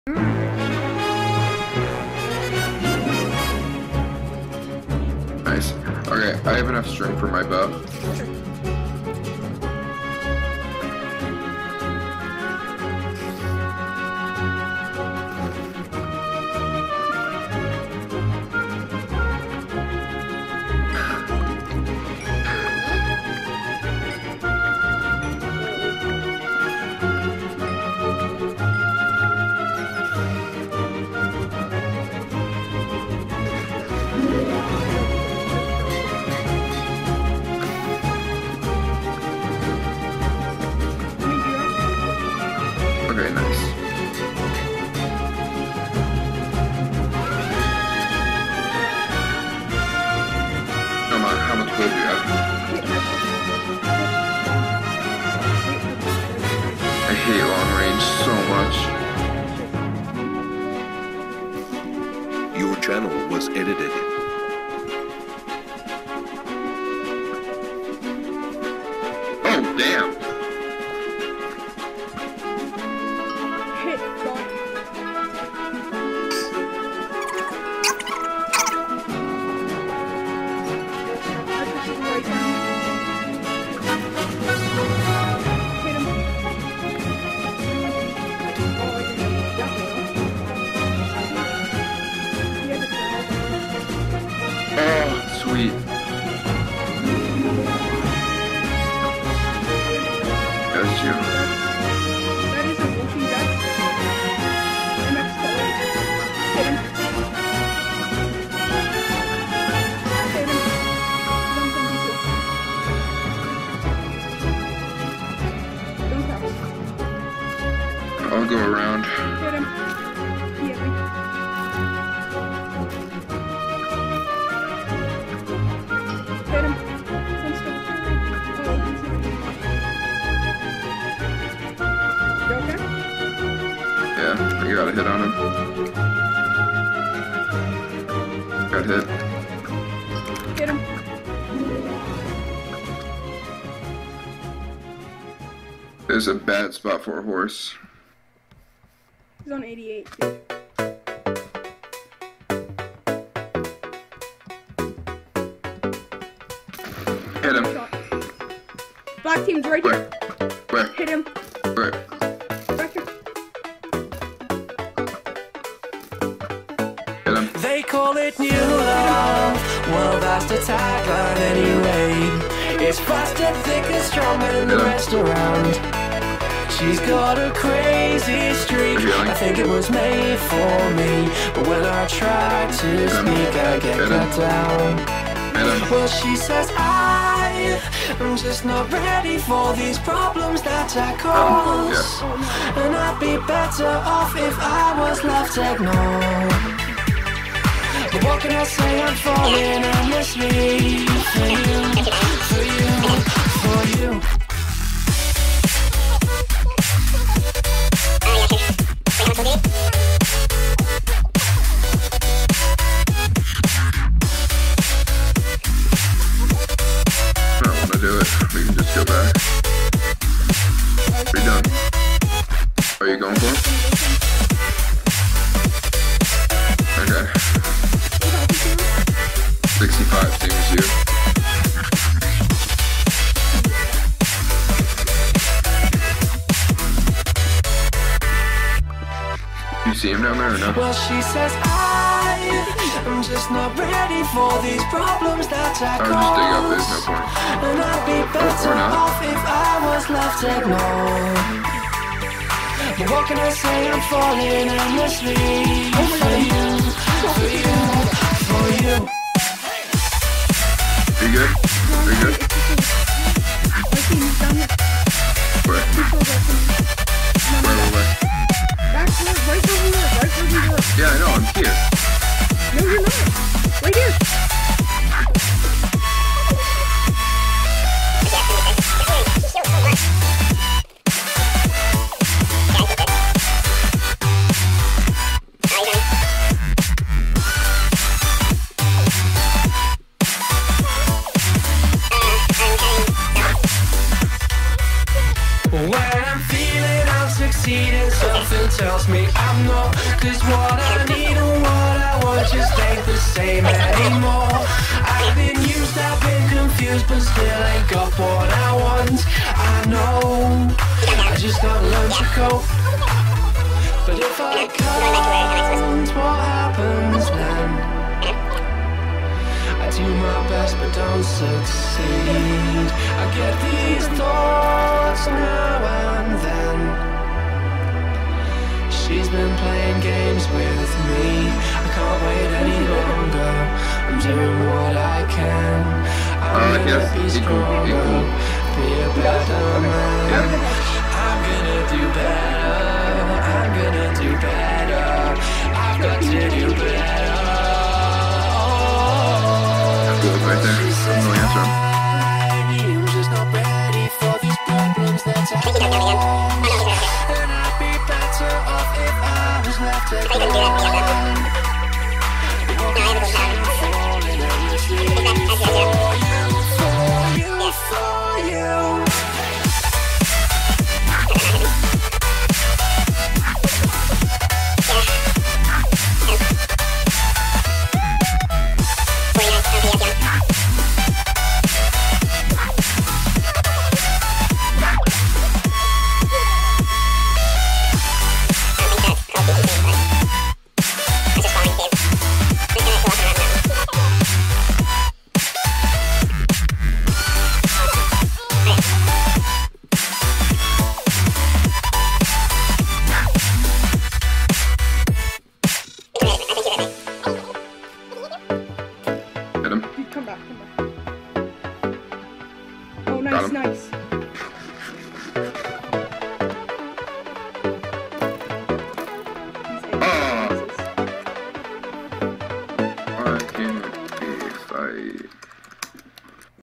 Joeylink's UHC Highlights #4 rTo2
Rto2 hosted by Greatkid. I was paired with kookleFTW and we did pretty good. Sorry Walshy about dat horse xdddd u were salty